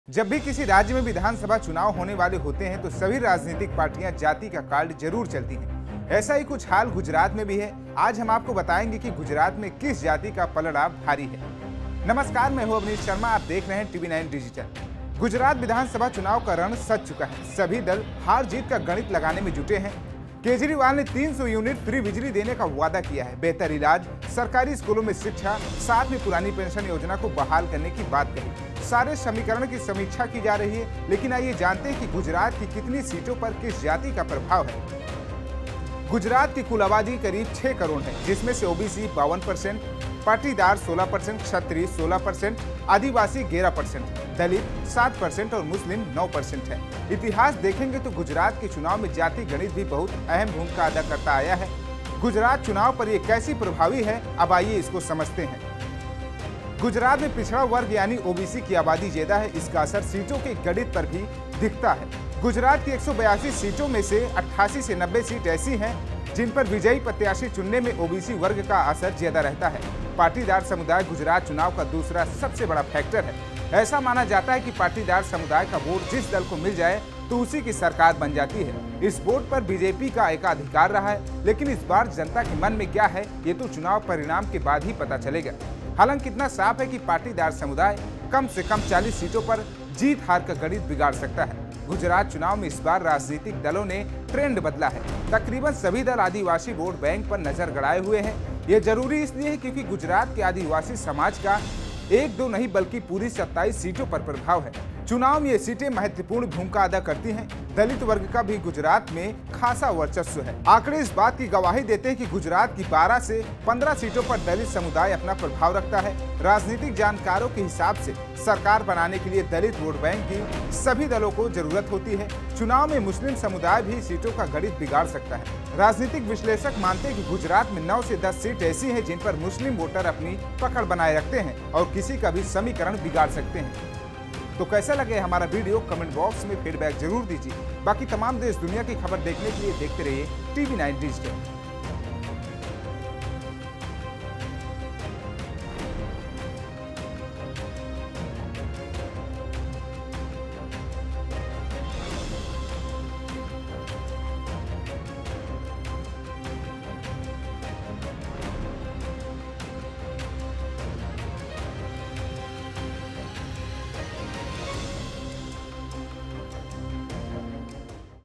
Gujarat Election : गुजरात विधानसभा चुनाव में राजनीतिक दलों ने झोंकी ताकत | BJP | Congress
गुजरात (Gujarat) में पिछड़ा वर्ग यानि OBC की आबादी ज्यादा है, इसका असर सीटों के गणित पर भी दिखता है, गुजरात की 182 सीटों में से 88 से 90 सीटें ऐसी हैं, जिन पर विजयी प्रत्याशी चुनने में OBC वर्ग का असर ज्यादा रहता है. पाटीदार समुदाय गुजरात चुनाव (Gujarat Election) का दूसरा सबसे बड़ा फैक्टर है, ऐसा माना जाता है कि पाटीदार समुदाय का वोट जिस दल को मिल जाए तो उसी की सरकार बन जाती है

Credit: National | AuneeshSharma | Editor/...